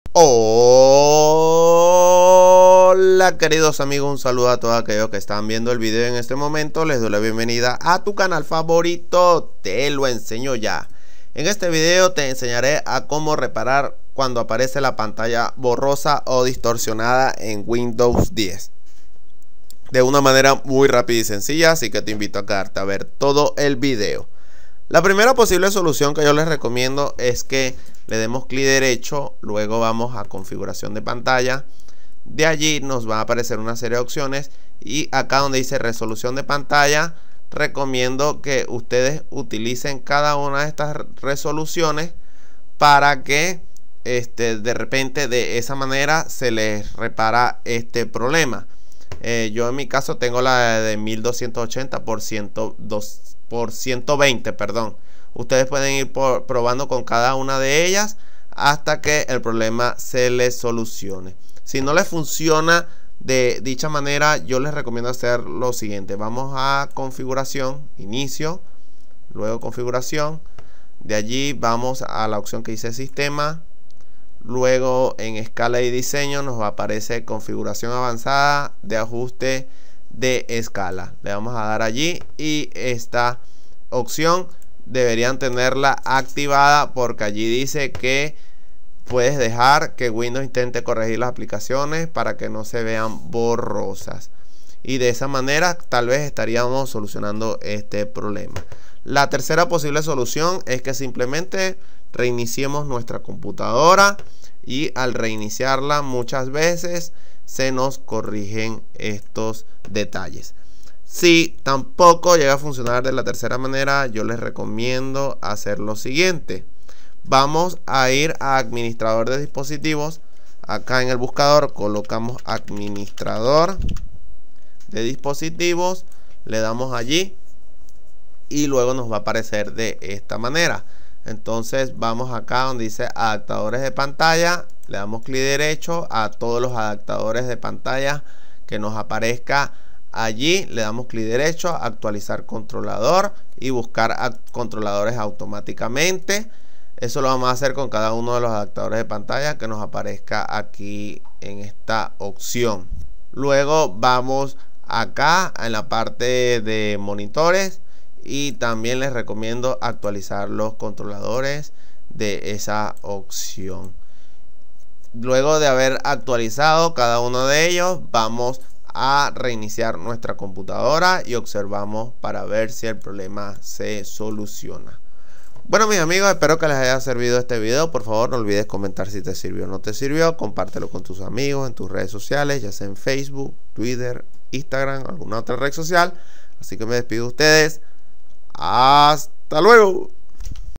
Pantalla Borrosa o Distorsionada en Windows 10 I 4 Soluciones 2025
En este video te enseñare a Como reparar Pantalla Borrosa o Distorsionada en Windows 10 de una manera rápida y sencilla que cualquier persona puede realizar

Videos Recomendados
💛 Como ACELERAR Celular